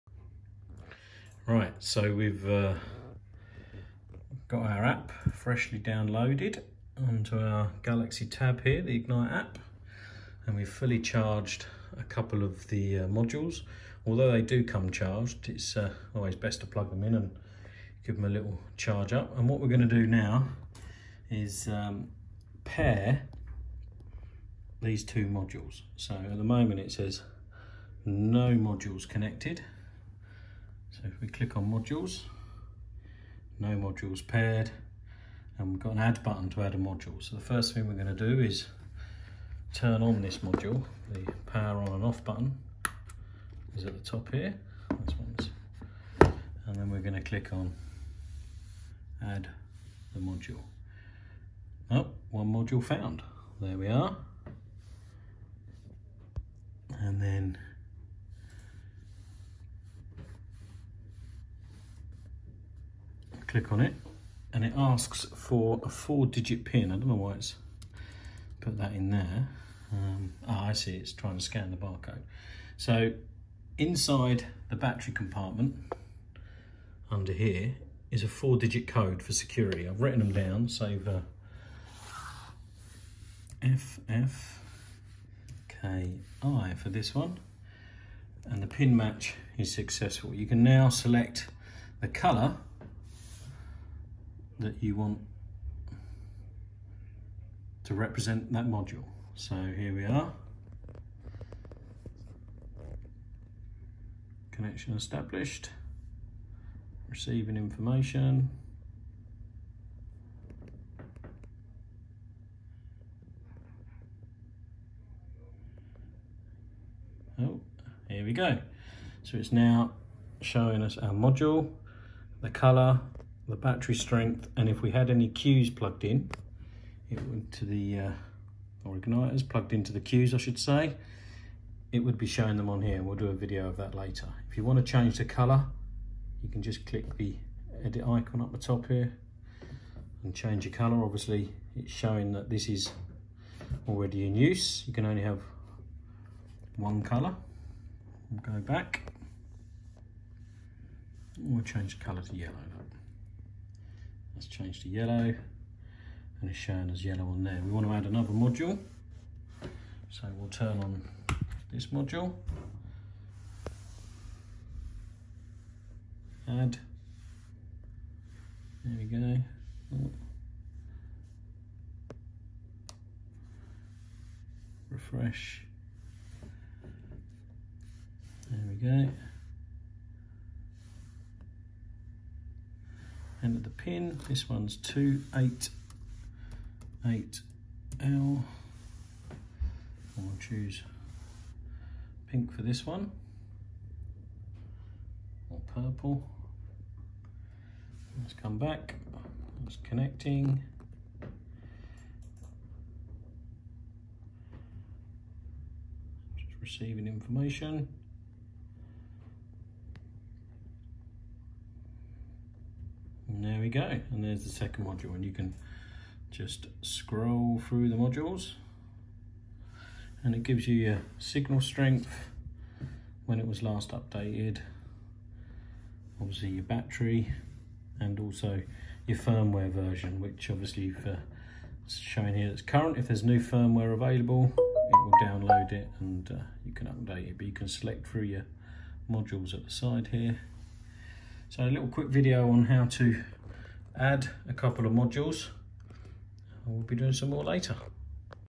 Pairing Ignite Modules
How to pair a couple of Ignite Firing System modules to the Ignite app.